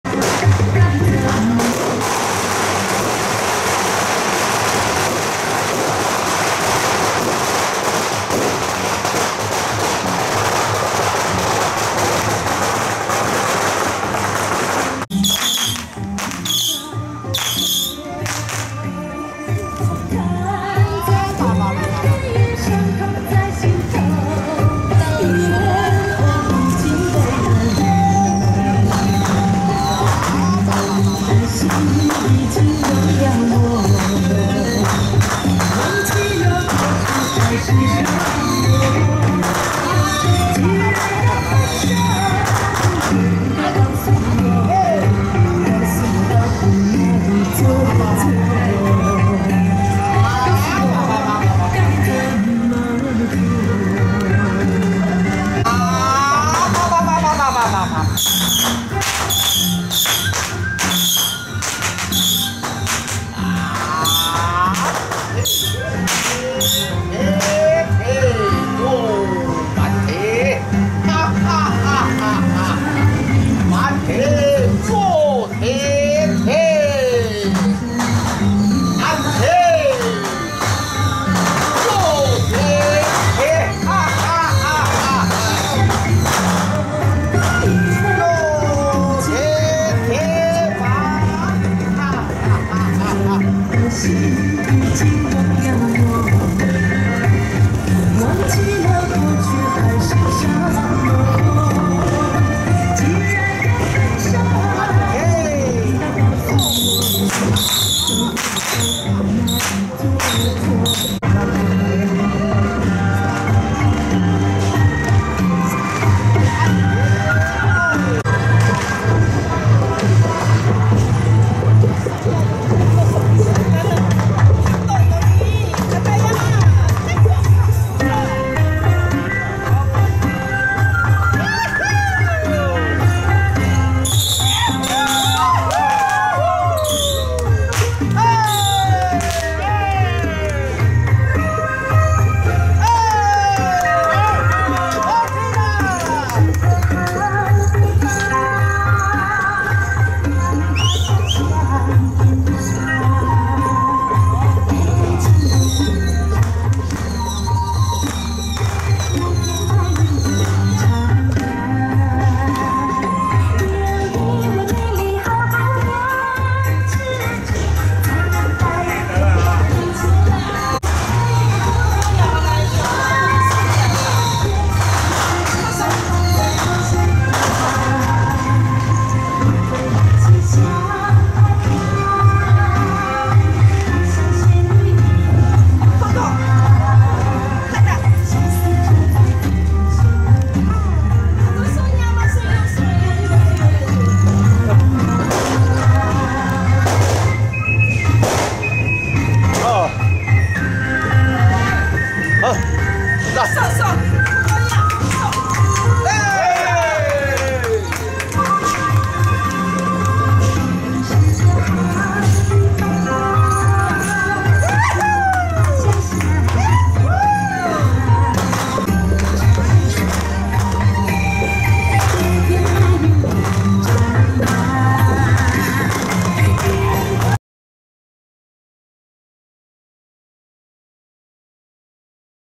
4/27 (六) 原住民老祖娘娘降駕就愛喝小米酒 (恆春鼻仔頭無極老祖宮)
4/27 (六) 屏東林邊放索安瀾宮天上聖母聖誕平安遶境大典